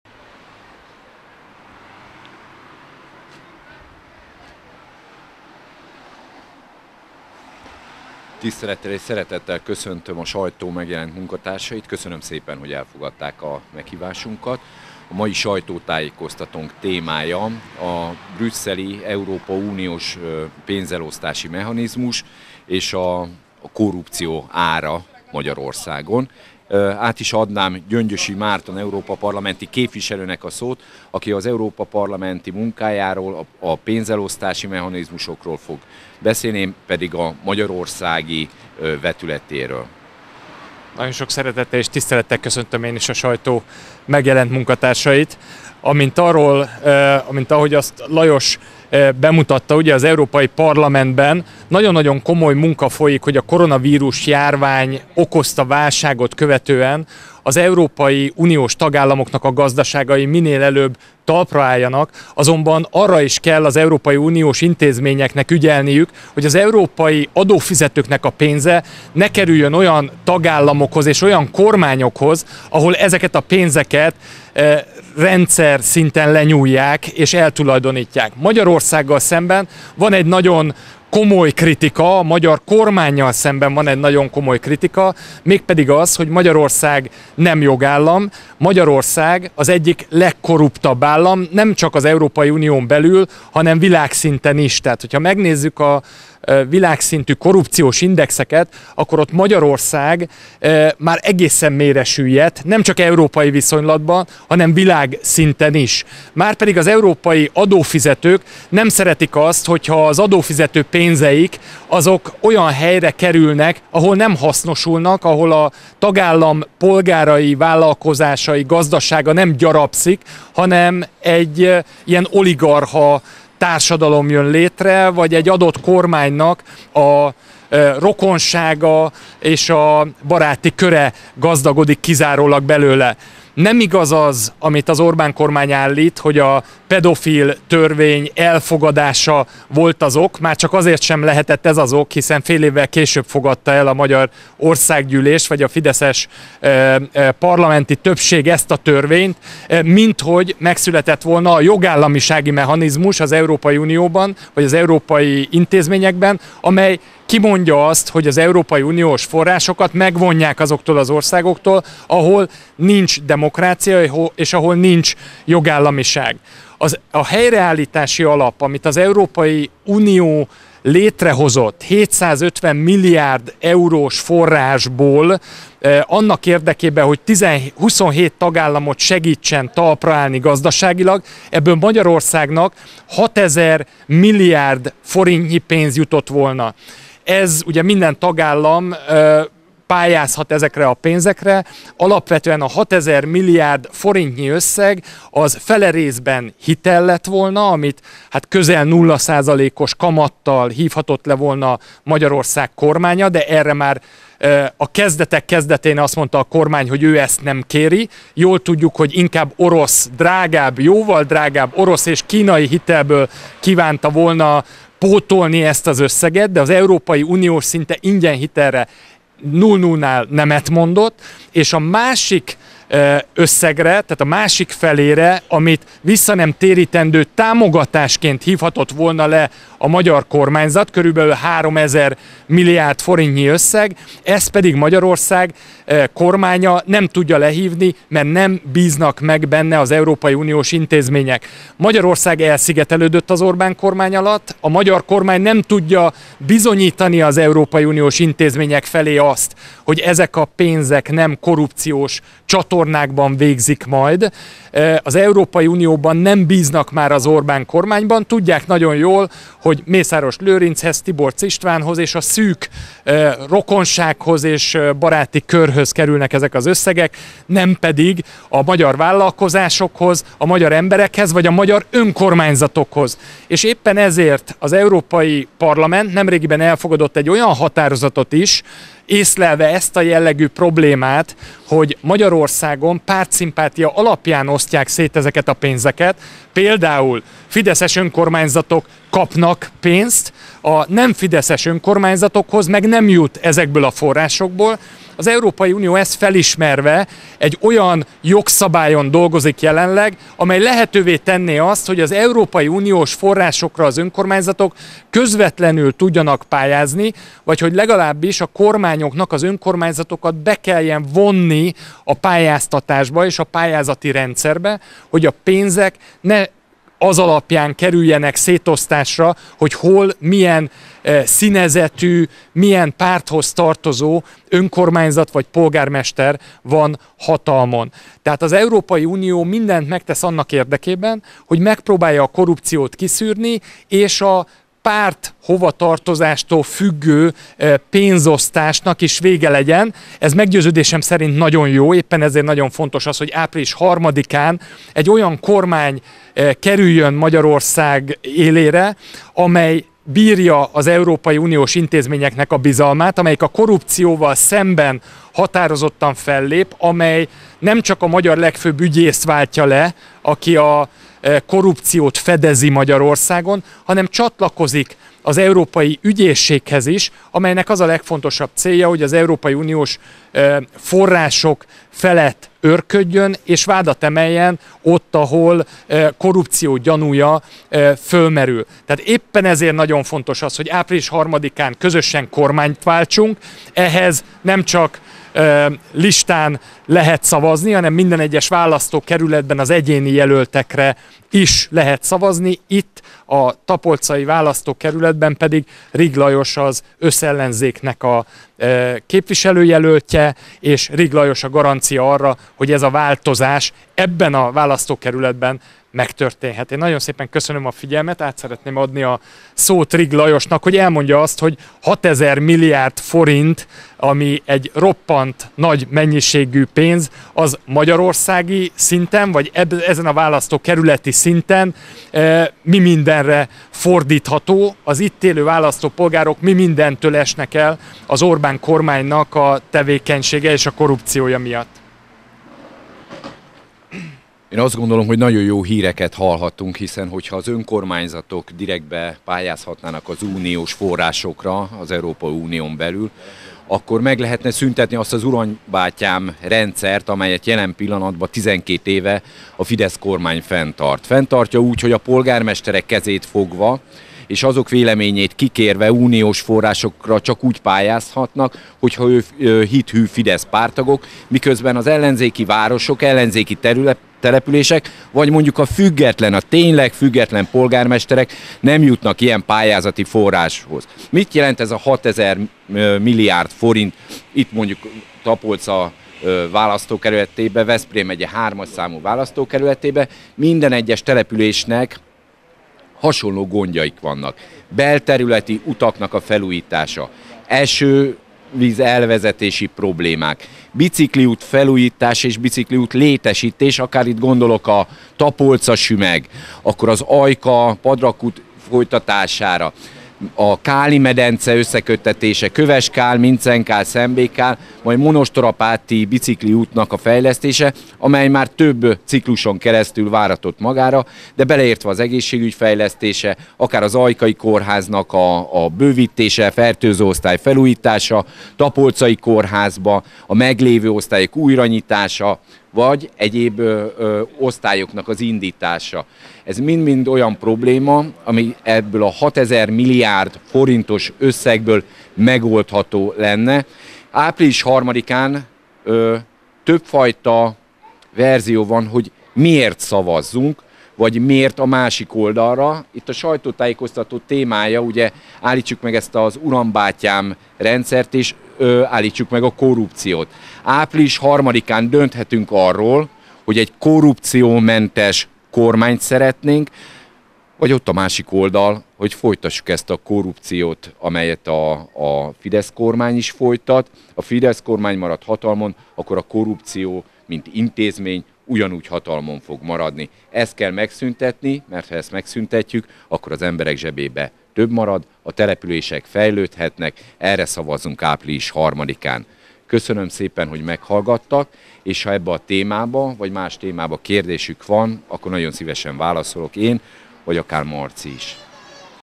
Rig Lajos és Gyöngyösi Màrton sajtótájékoztatója az EU-s döntésekkel kapcsolatban.
Közös sajtótájékoztatót tartott Tapolcán Rig Lajos (Jobbik) közös ellenzéki-jelölt és az őt támogató Gyöngyösi Márton (Jobbik) EP képviselő. Utóbbi elmondta, hogy az Európa Parlament elfogadta azt a határozati javaslatot, ami szerint a jövőben ki lehetne iktatni a politikai tényezőt a uniós támogatású pályázatok odaítélésénél, és az önkormányzatoknak kevesebb akadályt kellene leküzdeniük, hogy uniós forrásokhoz jussanak. - A különböző korrupciós felmérések igazolják, hogy Magyarország már világviszonylatban is a legkorruptabb államok közé tartozik – vezette fel mondandóját az EP képviselő.